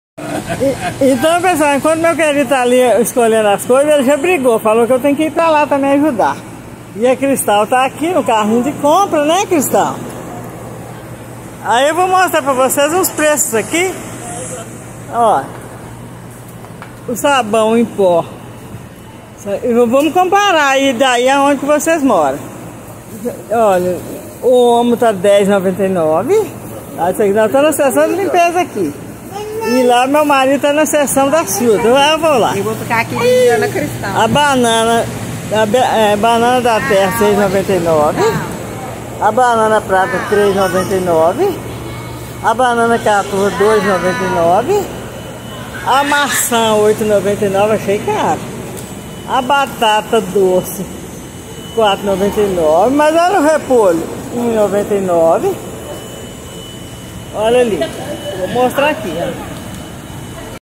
do Supermercado com Cristal. (1)
Nesses shorts mostro nós fazendo umas comprinhas para a virada do ano.